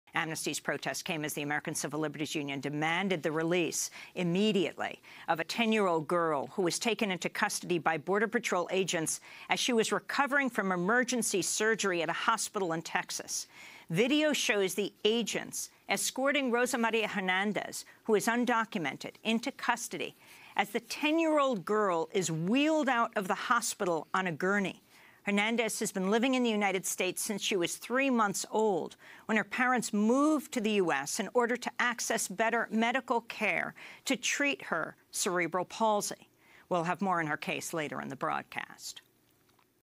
Unión Estadounidense por las Libertades Civiles exige la liberación de niña de 10 años indocumentada
Un video muestra cómo los agentes escoltan a Rosa María Hernández, que está indocumentada, para detenerla mientras la sacan del hospital en una camilla. Hernández vive en Estados Unidos desde los tres meses de edad, cuando sus padres se mudaron a ese país para acceder a mejor asistencia médica para tratar su parálisis cerebral.